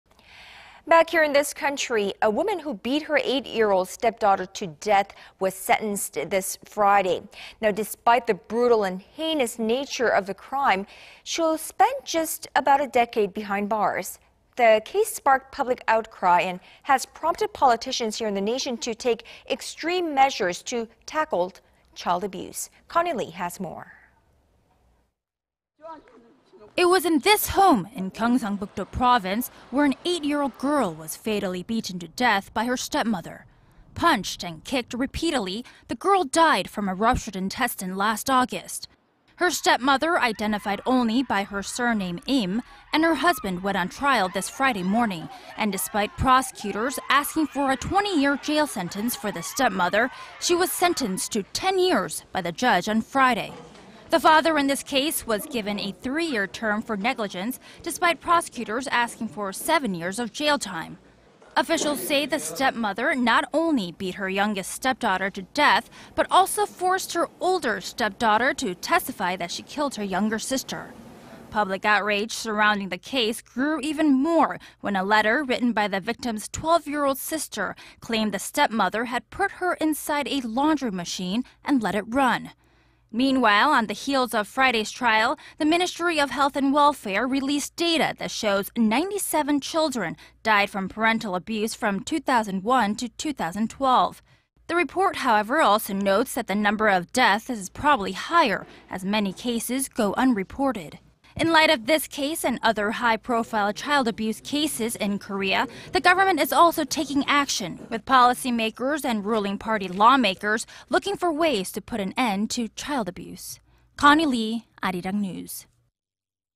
High-profile child abuse case begins
Here in Korea... a woman who beat her eight-year-old stepdaughter to death was sentenced this Friday.
Despite the brutal and heinous nature of the crime, she'll spend just ten years behind bars.
The case sparked public outcry,... and has prompted politicians here in the nation... to take extreme measures to tackle child abuse.
Connie Lee has more. It was in this home... in Gyeongsangbuk-do province... where an 8-year-old girl was fatally beaten to death by her stepmother.
Punched and kicked repeatedly... the girl died from a ruptured intestine last August.

Her stepmother,... identified only by her surname Lim,... and her husband... went on trial this Friday morning...
Despite prosecutors asking for a 20-year jail sentence for the stepmother,... she was sentenced to 10 years by the judge on Friday.
The father in this case... was given a 3-year term for negligence... despite prosecutors asking for 7 years of jail time.
Prosecutors say the stepmother... not only beat her youngest stepdaughter to death... but also... for forced her older stepdaughter to testify that she killed her younger sister.

Public outrage surrounding the case grew even more... when a letter... written by the victim's 12-year-old sister... claimed the stepmother had put her inside a laundry machine and let it run.
Meanwhile, on the heels of Friday's trial. the Ministry of Health and Welfare released data that shows 97 children died from parental abuse from 2001... to 2012.
The report, however, also notes that the number of deaths is probably higher...as many cases go unreported.

In light of this case and other high-profile child abuse cases in Korea... the government is also taking action... with policymakers and ruling party lawmakers looking for ways to put a stop to them.
Connie Lee, Arirang News.